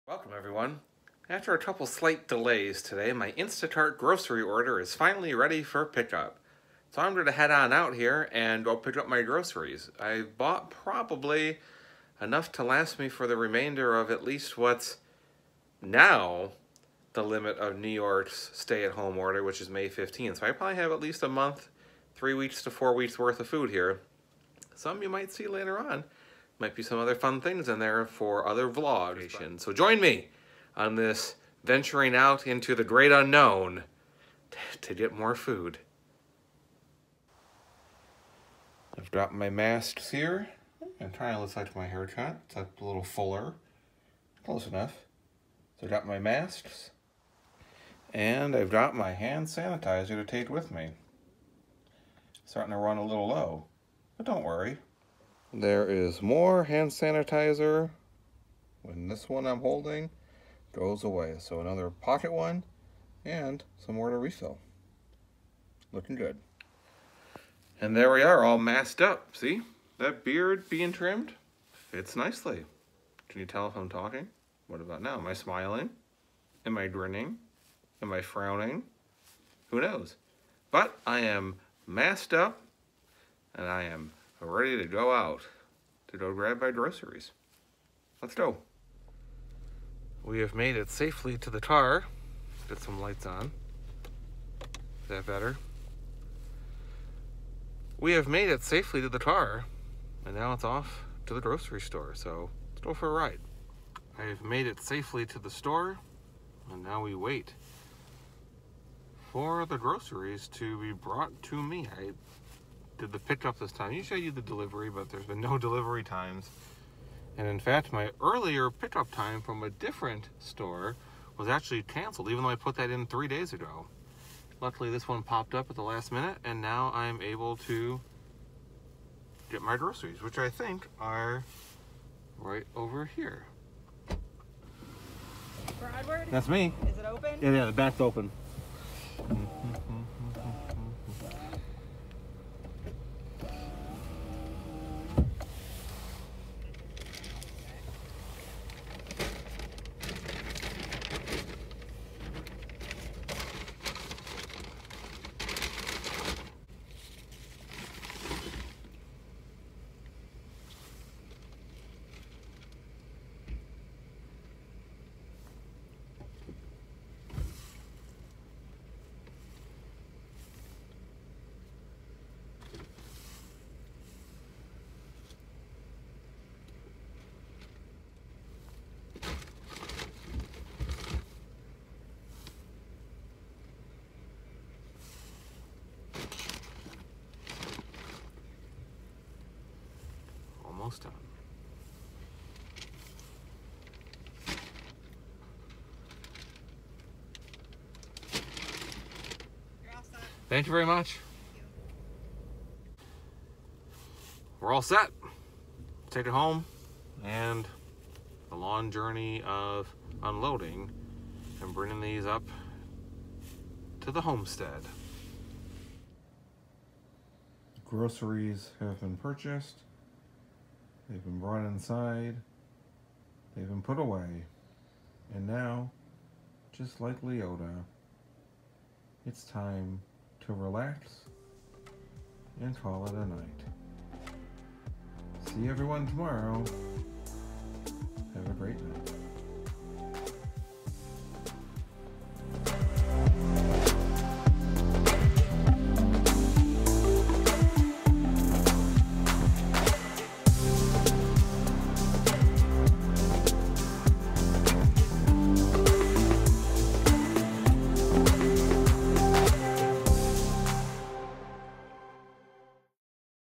Every Day at Home: Tops
It’s day four, and while it’s not technically a forage, it was time to restock the food supply. A quick look at grocery pick up.